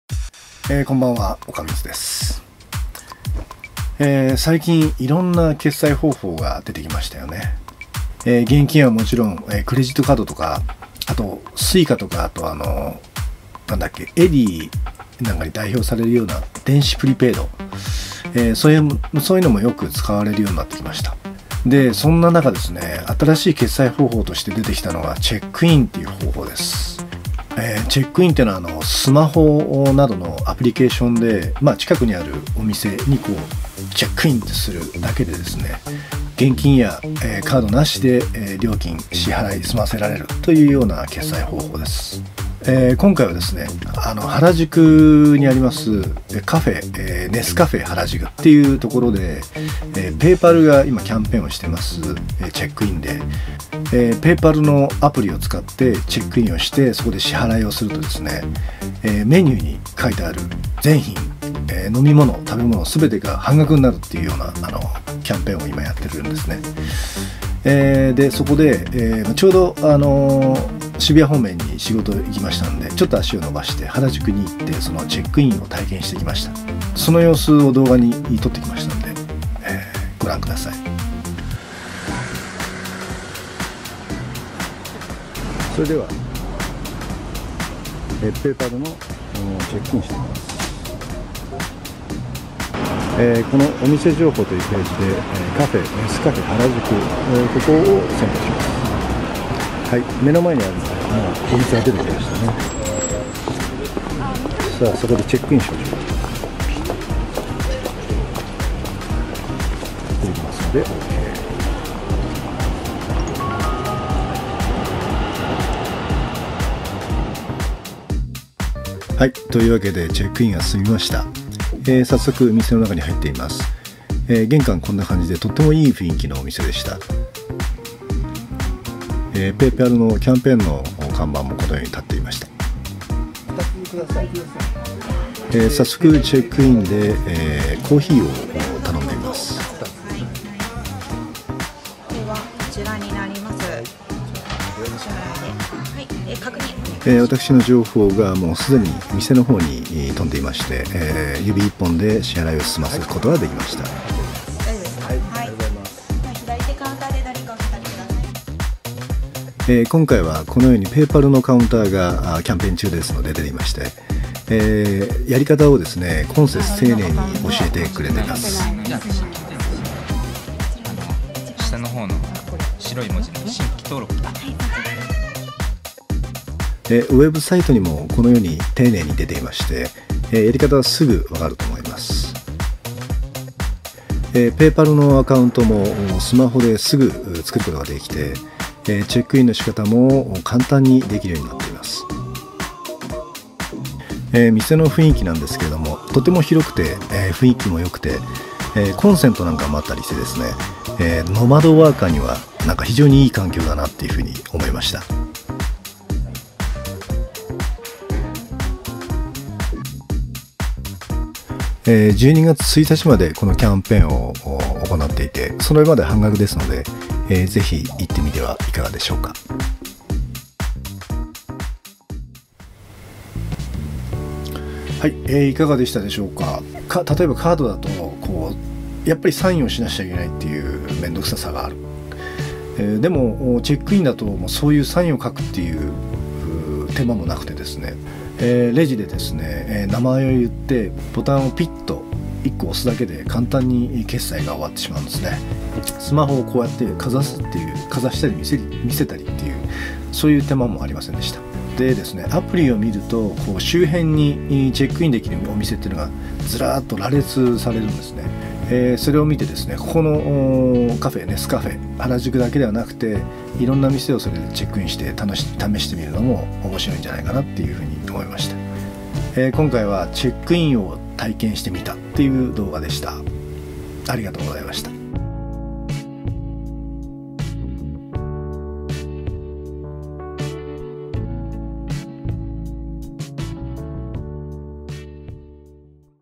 paypal ペイパルチェックインでコーヒーを飲んでみた Cafe Nescafe原宿
paypal_nescafe
便利な世の中になったモノです。